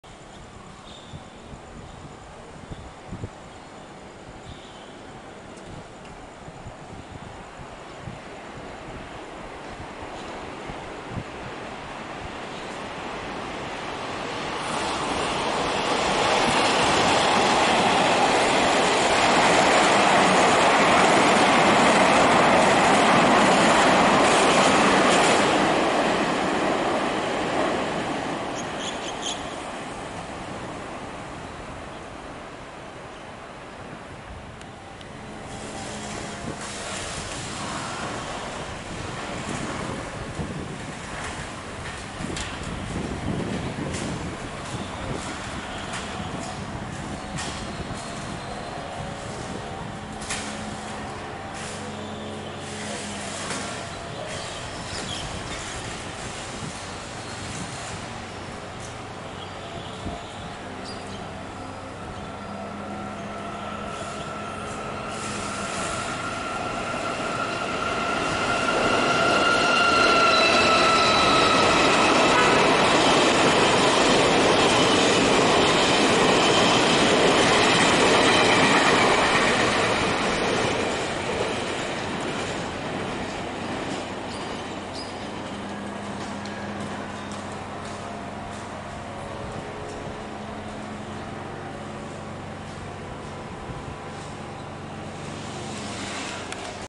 2019-02-22 橋頭車站2列莒光號通過紀錄
第1列:莒光號  754次  經南迴  臺東 - 彰化
第2列:莒光號  513次  經海線  七堵 - 潮州